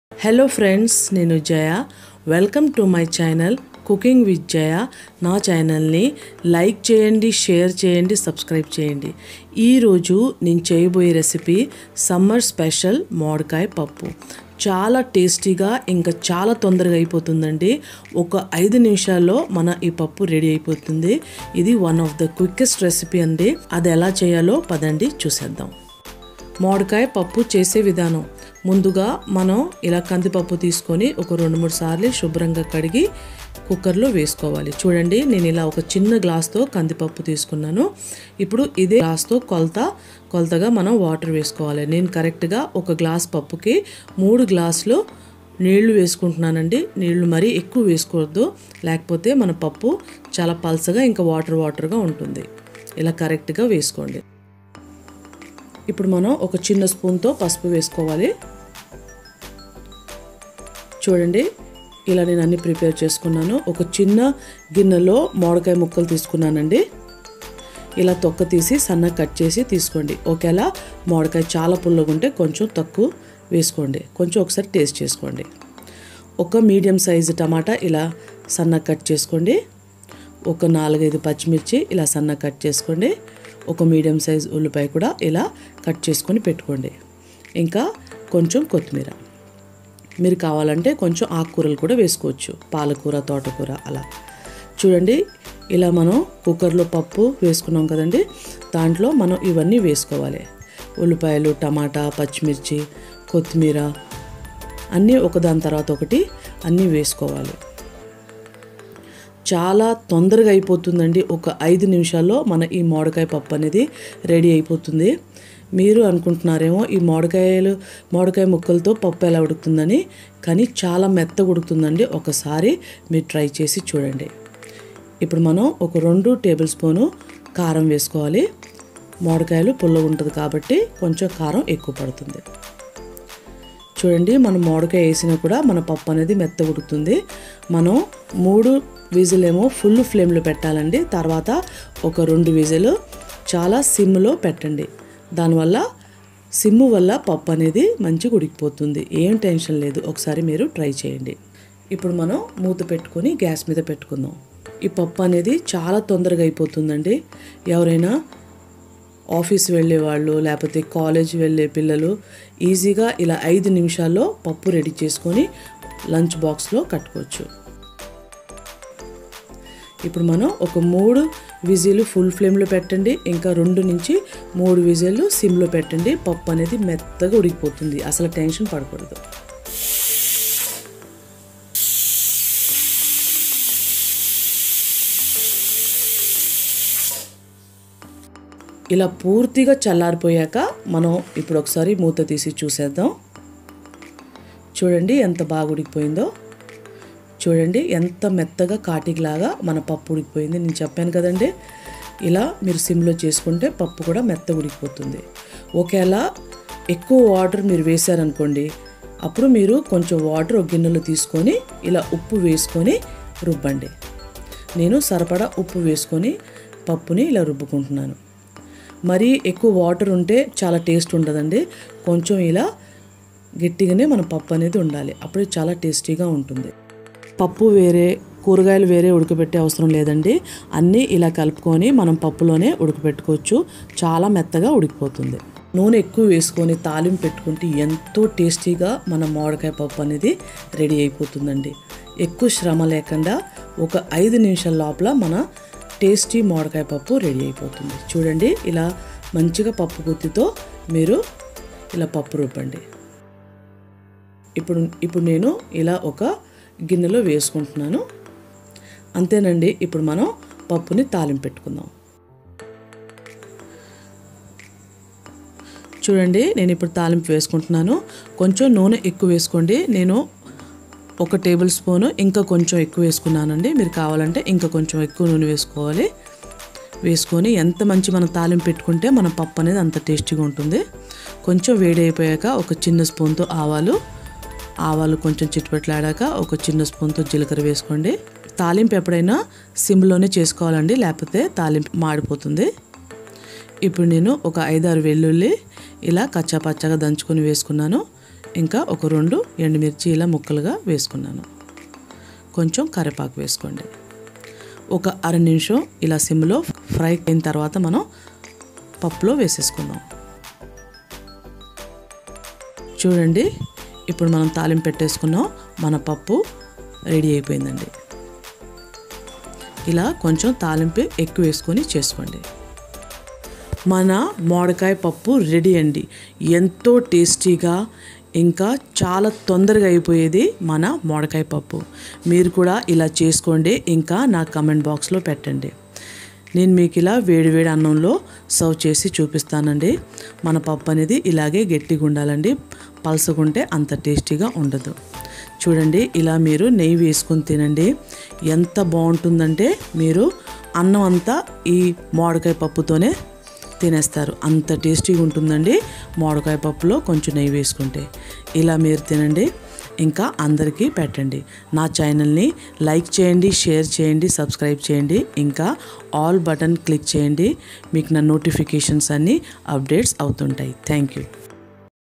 Mamidikaya Pappu Recipe in Telugu/Raw Mango Dal/Andhra Style/Cooking With Jaya
This Mamidikaya Pappu Recipe is very easy to make and delicious to eat.
Cooking With Jaya